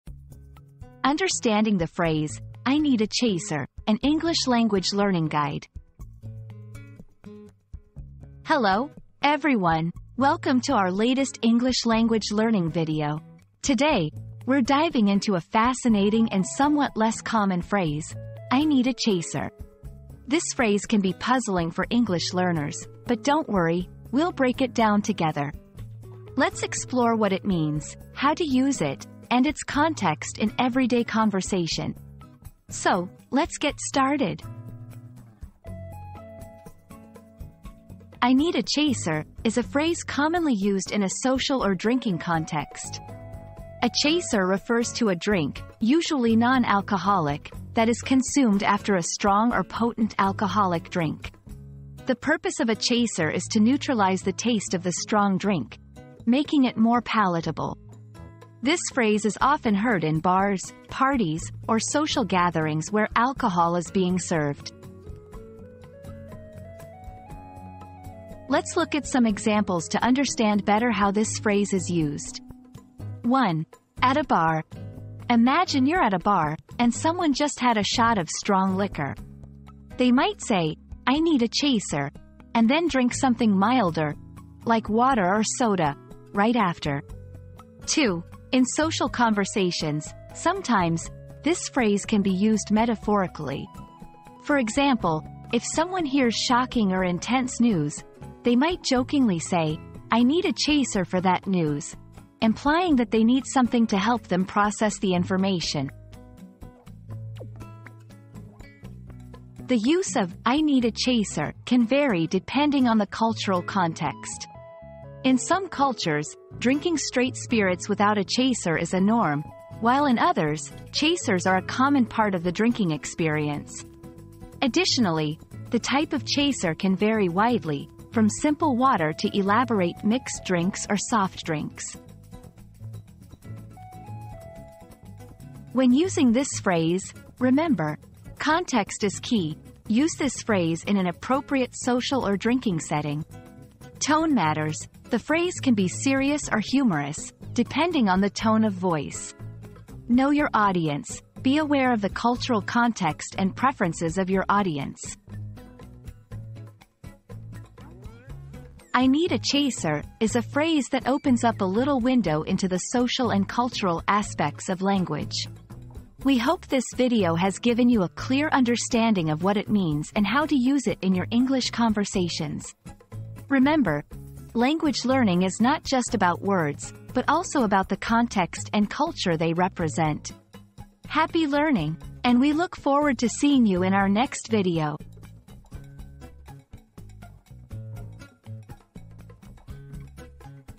Understanding the Phrase "I Need a Chaser": An English Language Learning Guide
00:00 • Introduction - Understanding the Phrase "I Need a Chaser": An English Language Learning Guide
00:37 • What Does "I Need a Chaser" Mean?
01:07 • Examples of Using "I Need a Chaser"
01:49 • Cultural Context and Variations
02:15 • Tips for Using "I Need a Chaser"